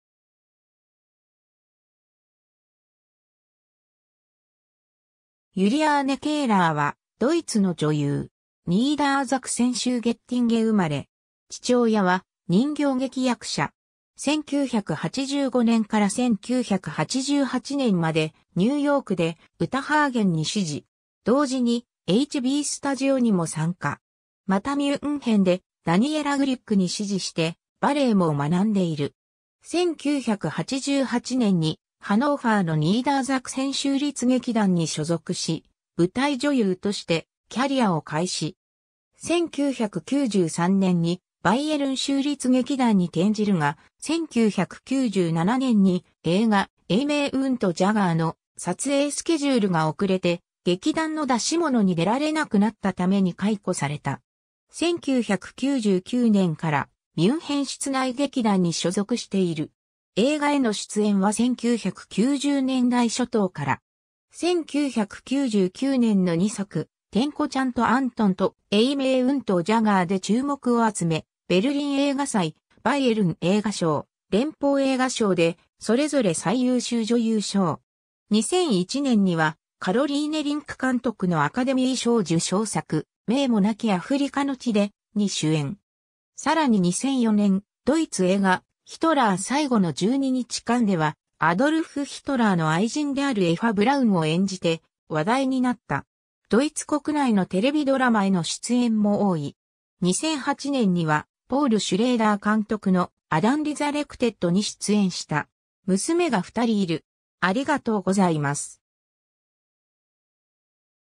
ユリアーネ・ケーラー
ユリアーネ・ケーラー（Juliane Köhler、1965年8月6日 - ）はドイツの女優。
 ニーダーザクセン州ゲッティンゲン生まれ。
父親は人形劇役者。
1985年から1988年までニューヨークでウタ・ハーゲンに師事。
同時にH.B.スタジオにも参加。
またミュンヘンでダニエラ・グリュックに師事してバレエも学んでいる。
1988年にハノーファーのニーダーザクセン州立劇団に所属し、舞台女優としてキャリアを開始。
1993年にバイエルン州立劇団に転じるが、1997年に映画『Aimée und Jaguar』の撮影スケジュールが遅れて劇団の出し物に出られなくなったために解雇された。
1999年からミュンヘン室内劇団に所属している。
 映画への出演は1990年代初頭から。
1999年の2作、『点子ちゃんとアントン』と『Aimée und Jaguar』で注目を集め、ベルリン映画祭、バイエルン映画賞、連邦映画賞でそれぞれ最優秀女優賞。
2001年にはカロリーネ・リンク監督のアカデミー賞受賞作『名もなきアフリカの地で』に主演。
さらに2004年、ドイツ映画『ヒトラー 〜最期の12日間〜』では、アドルフ・ヒトラーの愛人（後に妻）であるエファ・ブラウンを演じて話題になった。
ドイツ国内のテレビドラマへの出演も多い。
2008年にはポール・シュレイダー監督の『Adam Resurrected』に出演した。
 娘が2人いる。